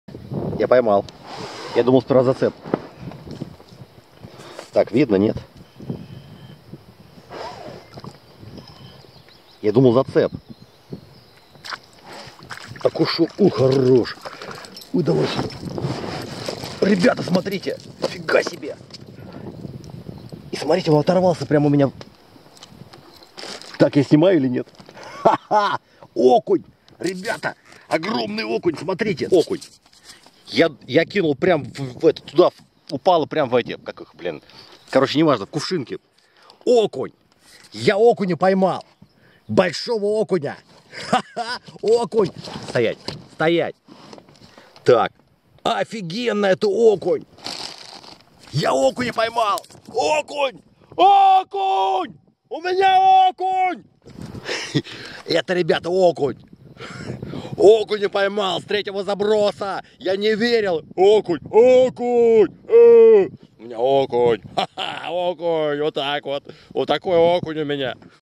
Окунь. Это ОКУНЬ!!!
Окунь был пойман в процессе вечерней рыбалки на озере малое Юринское (Смоленская обл.)
Такой всплеск эмоций - результат предшествующих событий...
Полный фильм о походе на малое Юринское озеро, рыбалке, ловле окуня на спиннинг и т.д. ...следующим фильмом на моем канале.